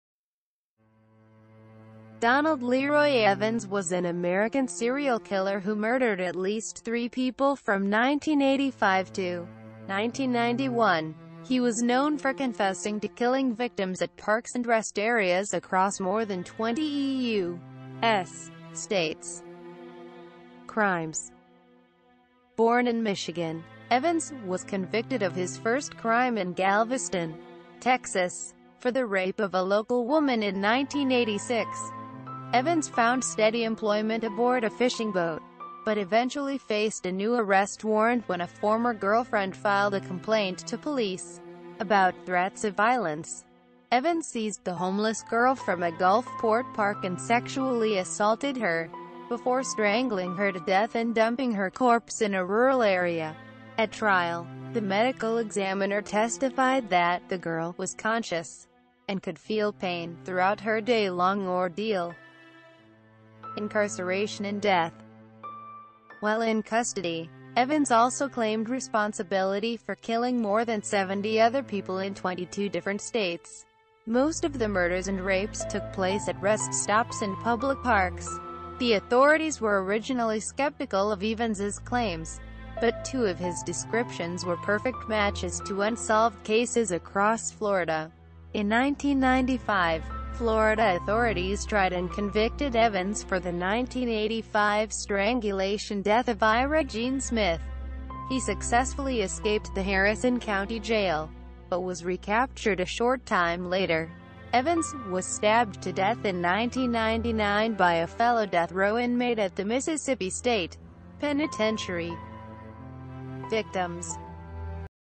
Donald Leroy Evans Documentary
Donald Leroy Evans  was an American serial killer who murdered at least three people from 1985 to 1991. He was known for confessing to killing victims at parks and rest areas across more than twenty U.S. states.